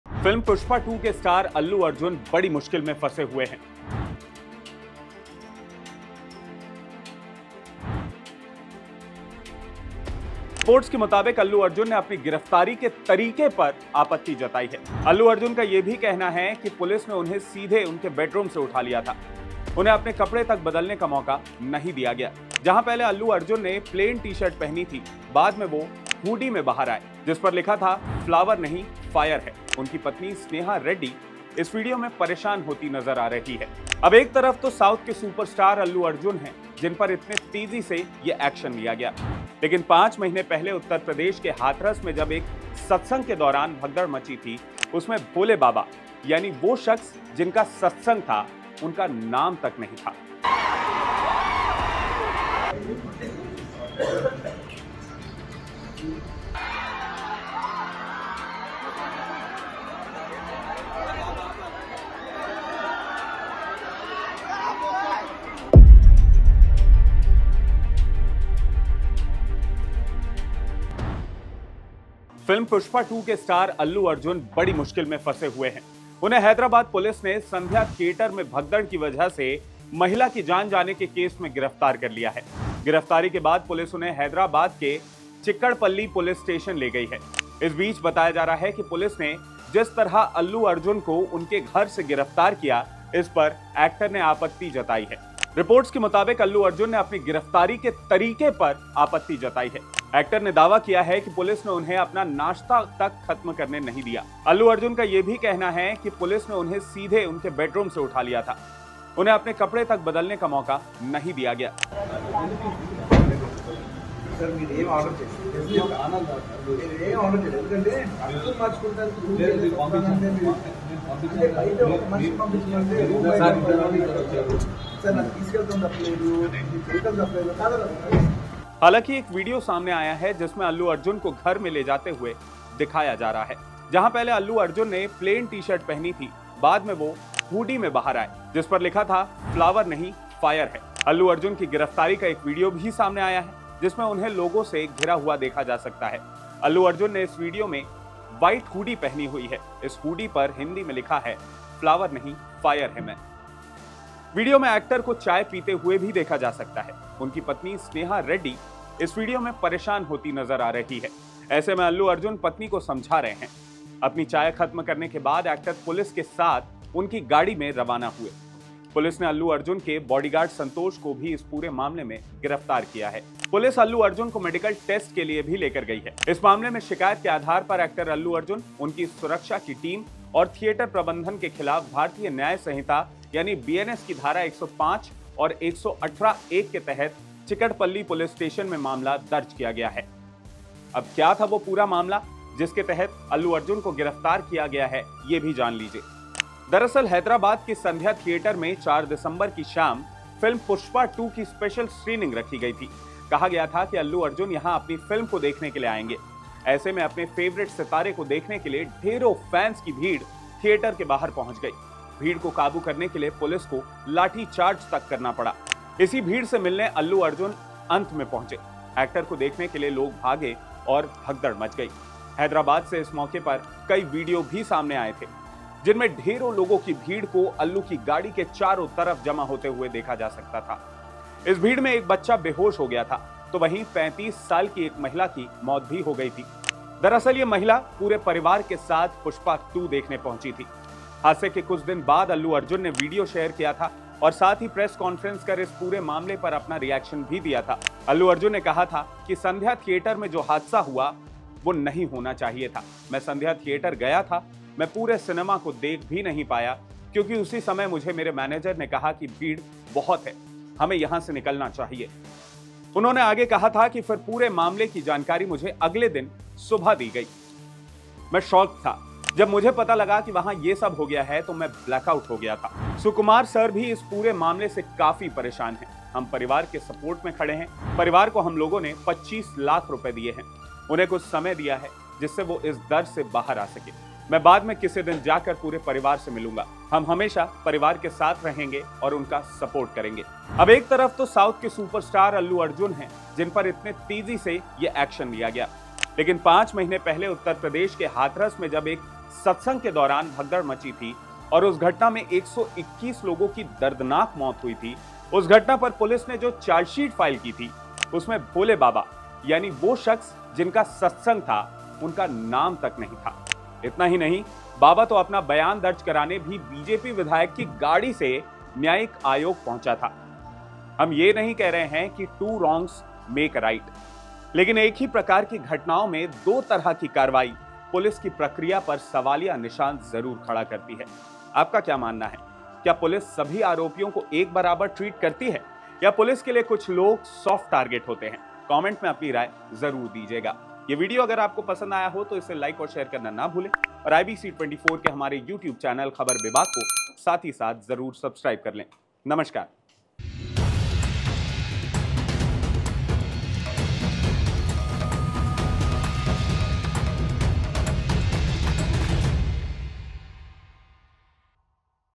Allu Arjun को तुरंत किया गिरफ्तार, पर Hathras वाले Bhole Baba पर क्यों नहीं गिरी गाज?
Allu Arjun को तुरंत किया गिरफ्तार, पर Hathras वाले Bhole Baba पर क्यों नहीं गिरी गाज? | Khabar Bebak | IBC24 के Khabar Bebak में आपका स्वागत है